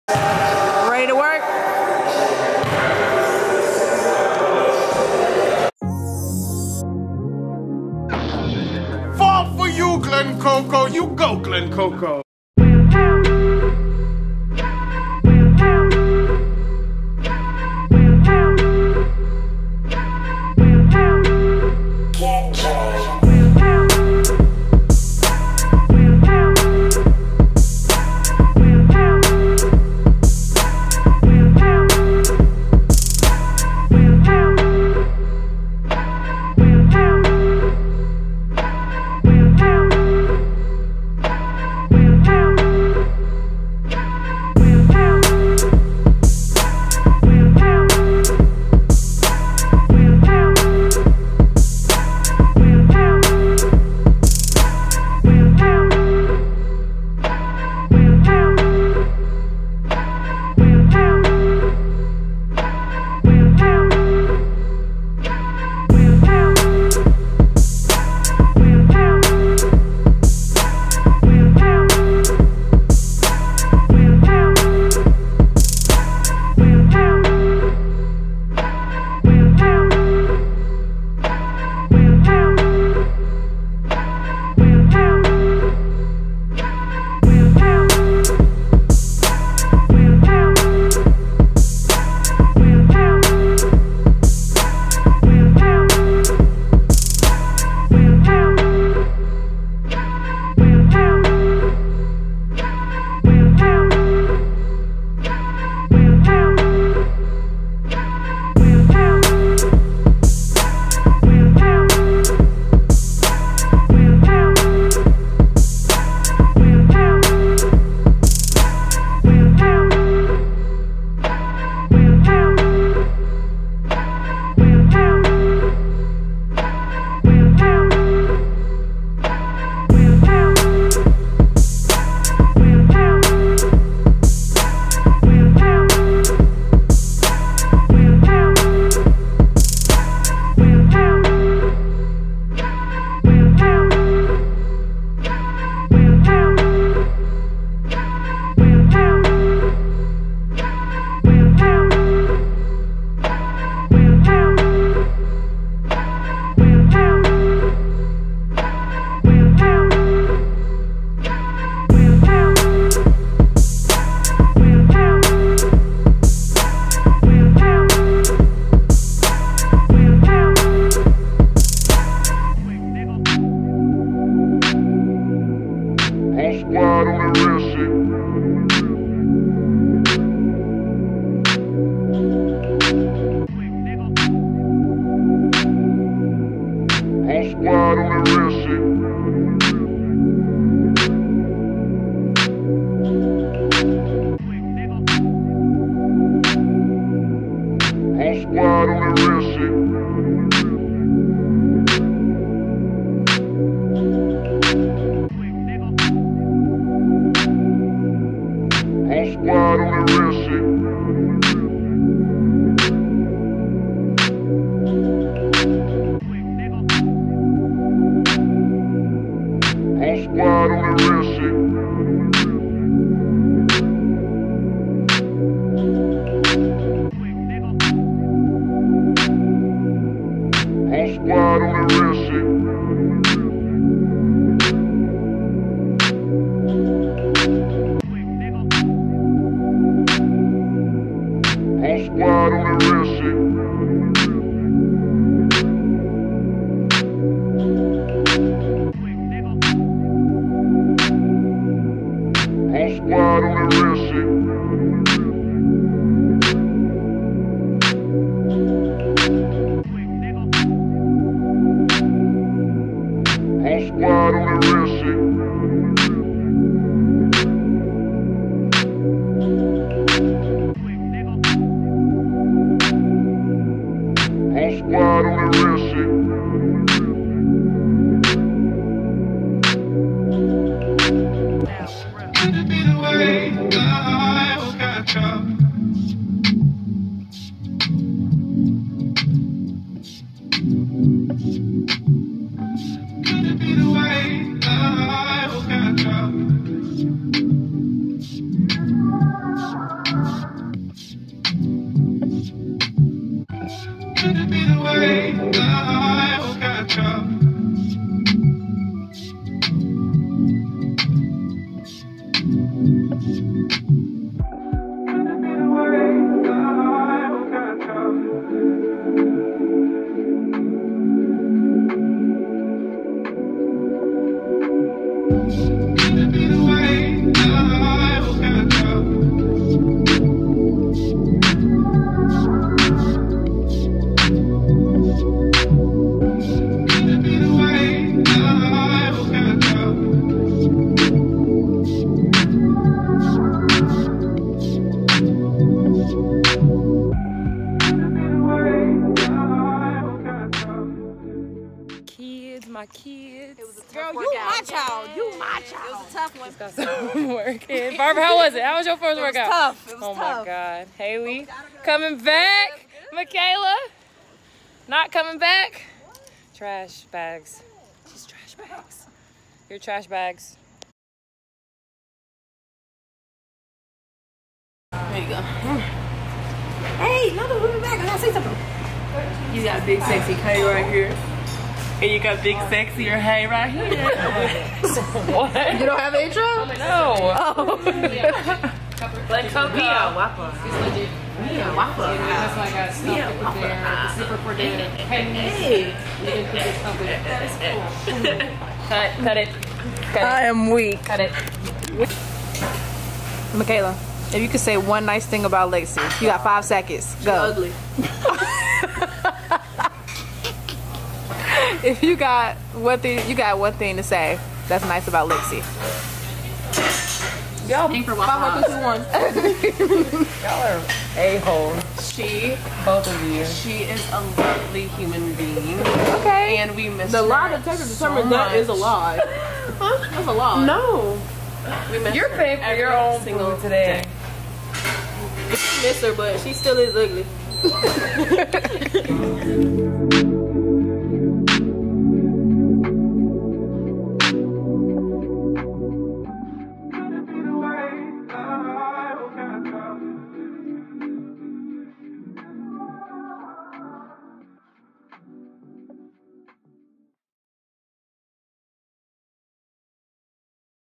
Workout Video | Lexie Brown
the good, the bad, and the ugly. I missed being on the court with my sisters. So many workout videos have only perfect reps...that's not reality. Especially not with me LOL.

FYI - Haley and Mikayla are COMING BACK THIS SEASON!! Can't wait to watch them!! Make sure you guys tune into their season this year.